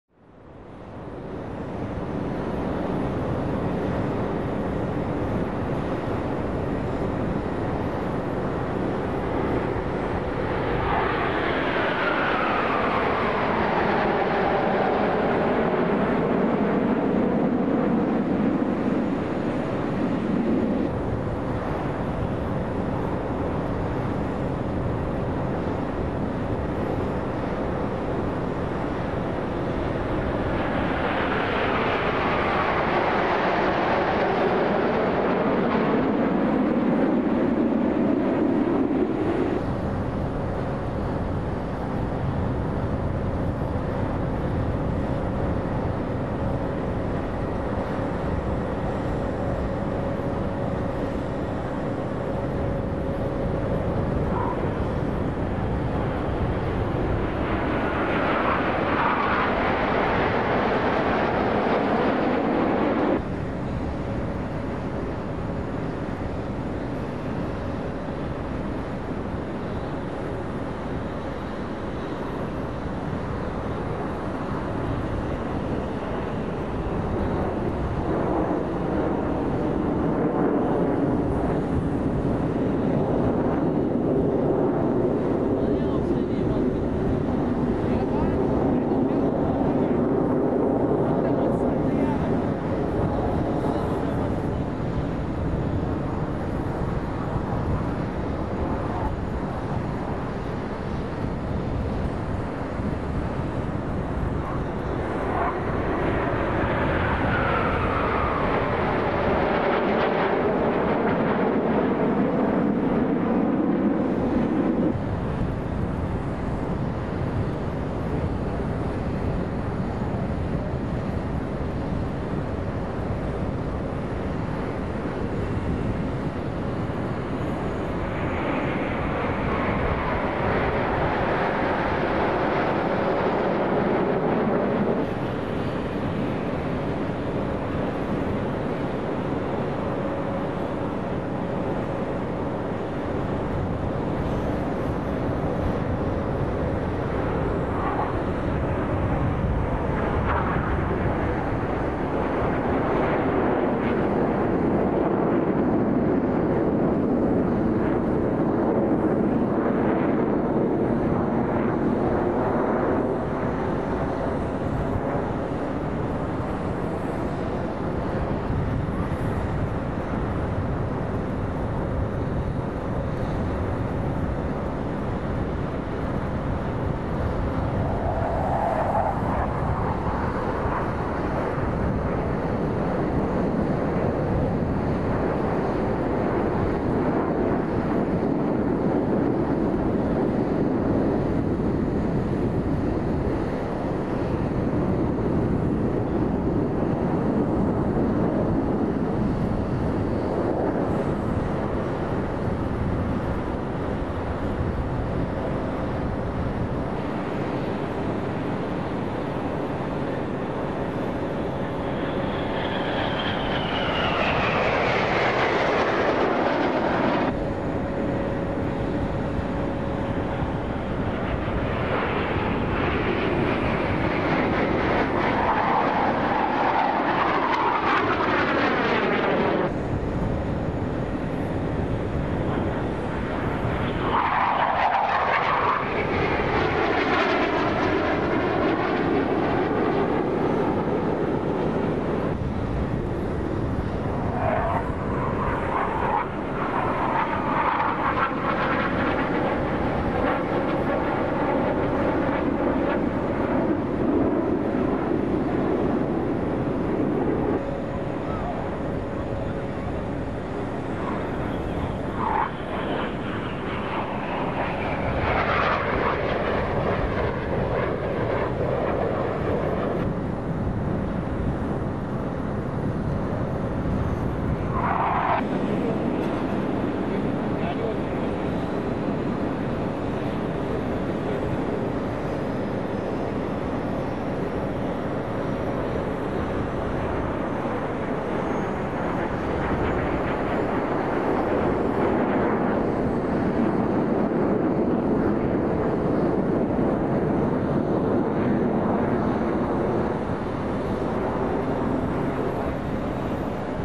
Blue Foam Extra shots: Indian Air Force Surya Kiran at Dubai Airshow 2021
Some shots that didn't make the Dubai Airshow 2021 video cut, uploaded for your viewing pleasure.